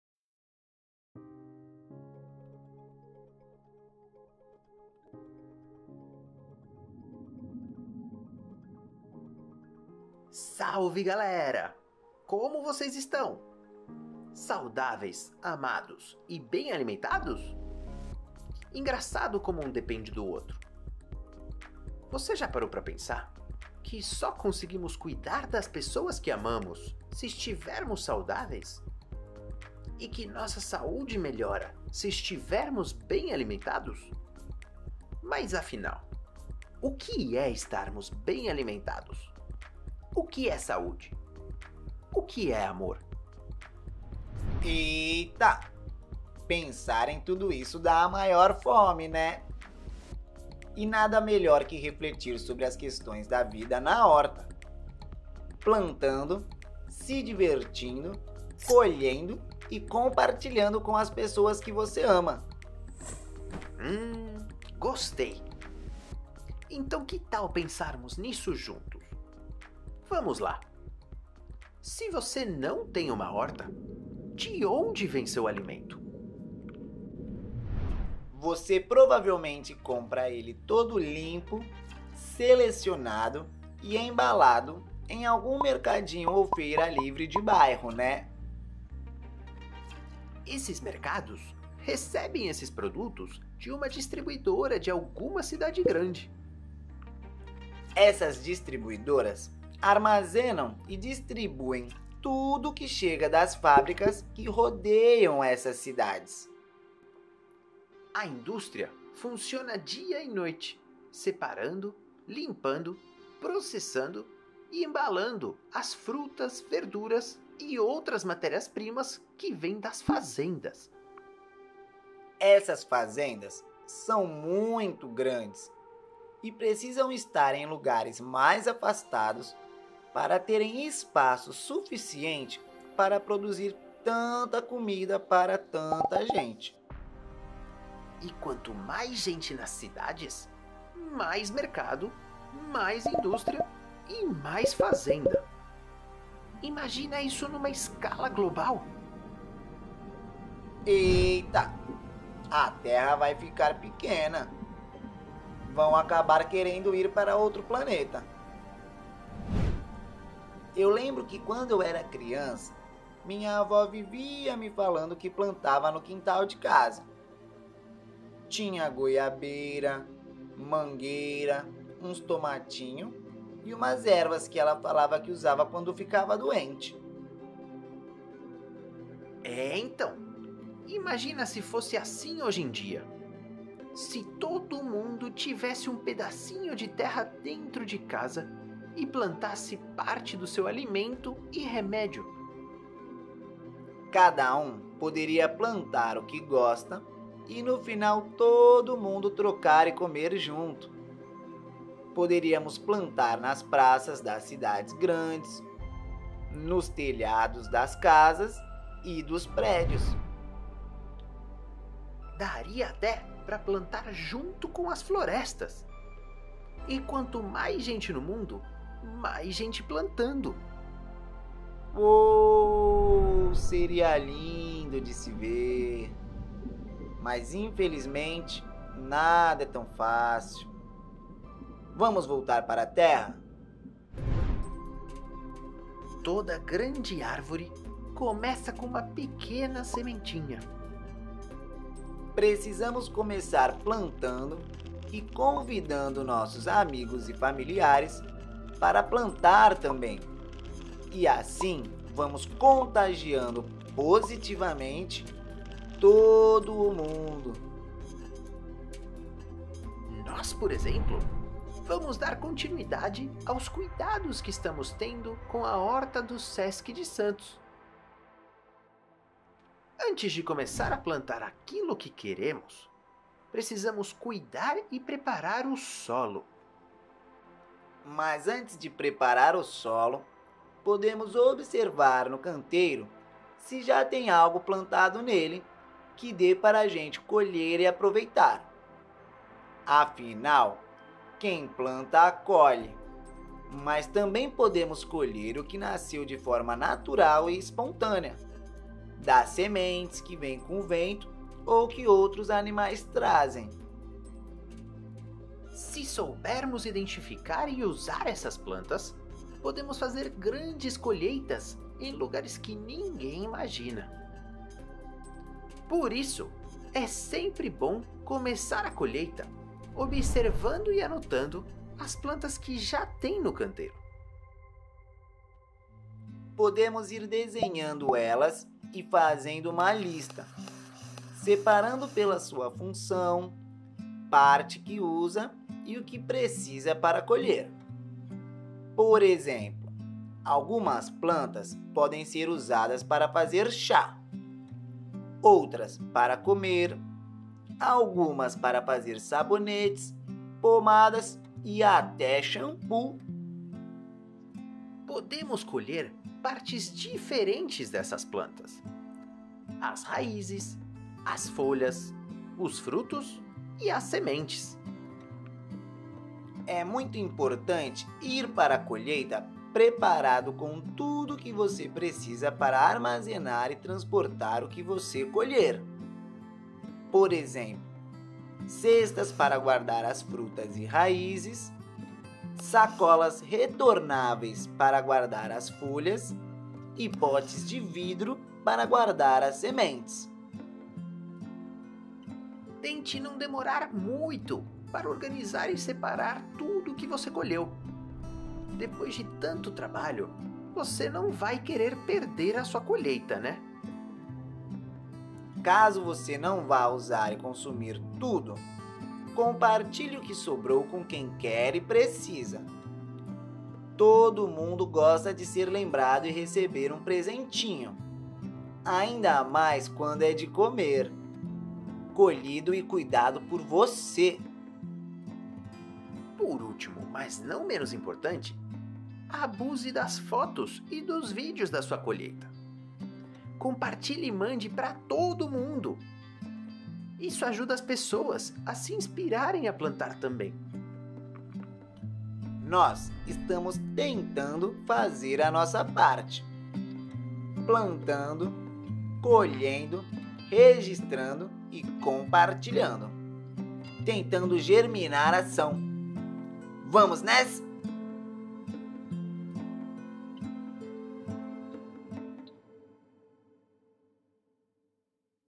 Florestar: Recuperar e Plantar - Colheita
Os meses de quarentena imposta pela Pandemia de Covid-19 fizeram com que áreas verdes, hortas e canteiros tivessem seus cuidados interrompidos. Com a possibilidade do retorno gradual, será necessário recuperar o cuidado com essas áreas. Porém, ausência de cuidado não significa ausência de vida! Longe dos nossos olhos, esses espaços continuam a se desenvolver, seguindo sua dinâmica natural.

Neste minicurso composto por cinco videoaulas, o Coletivo Germinação convida o público a se manter ativo e em contato com espaços plantados e áreas verdes durante o isolamento social, apresentando técnicas e possibilidades de planejamento e manejo desses espaços em recuperação.

Colheita: apresenta a importância de começar a manutenção pela colheita do que brotou espontaneamente no espaço a ser renovado. Quais são os melhores dias, a melhor hora e como realizar a colheita.